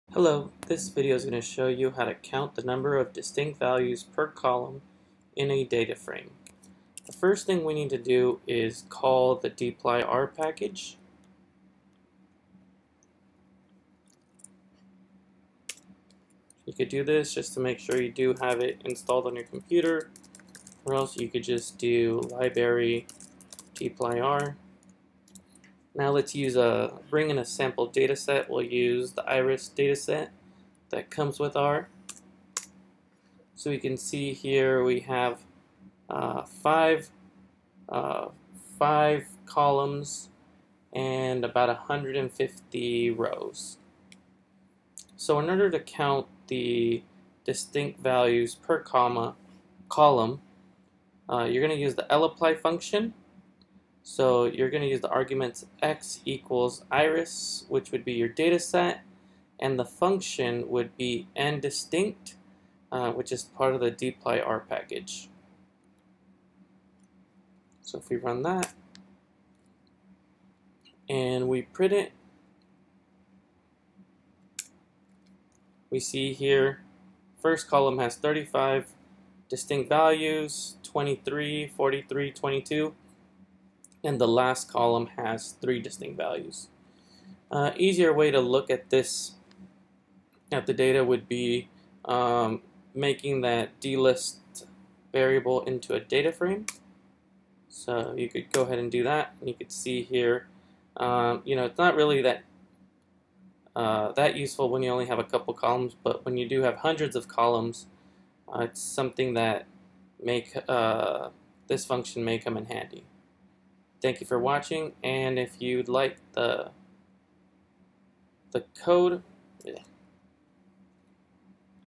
Learning R: 27 Counting Distinct Values per Column in DataFrame
This video tutorial will show you how to count distinct values for every column in a Data Frame.